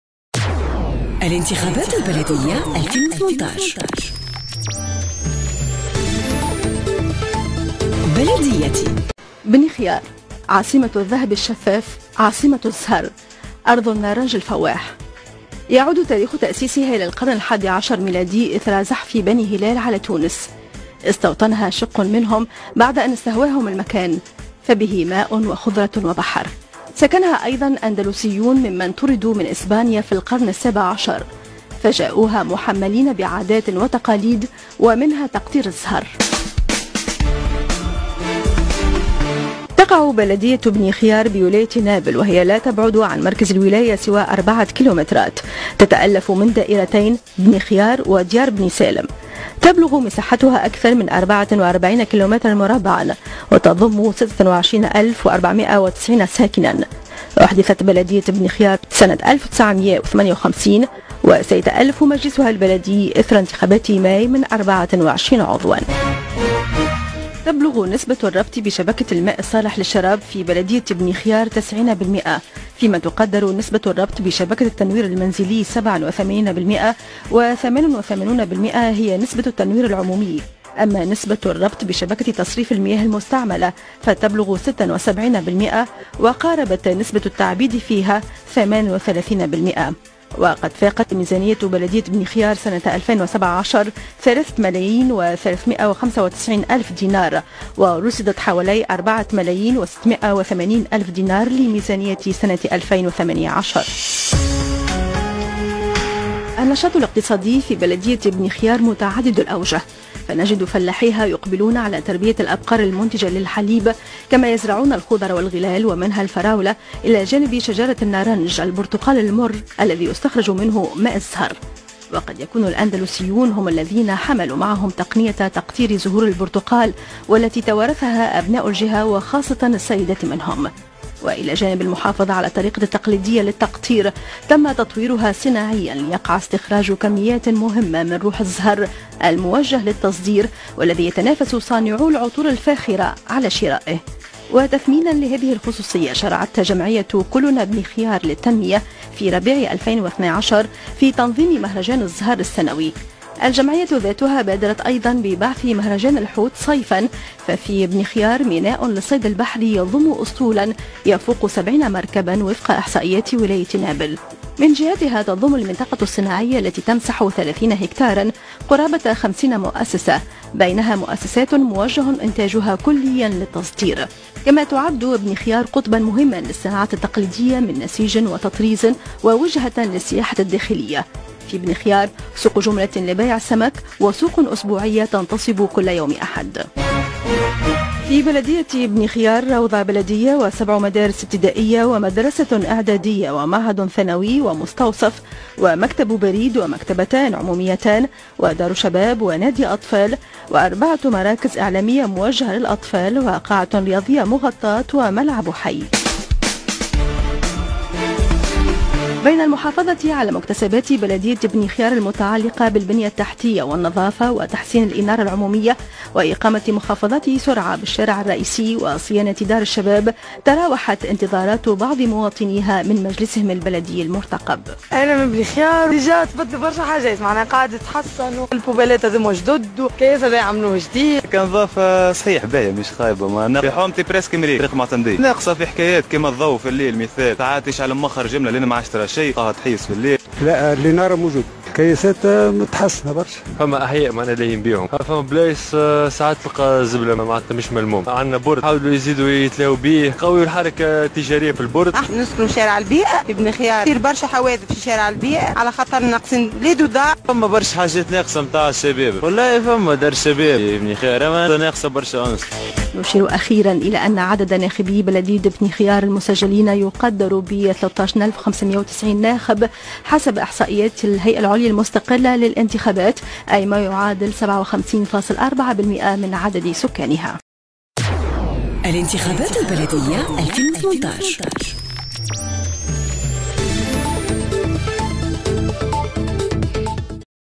بلديتي: بلدية بني خيار
بلديتي: التعريف بالبلديات في إطار الأنتخابات البلدية 2018 للجمهورية التونسية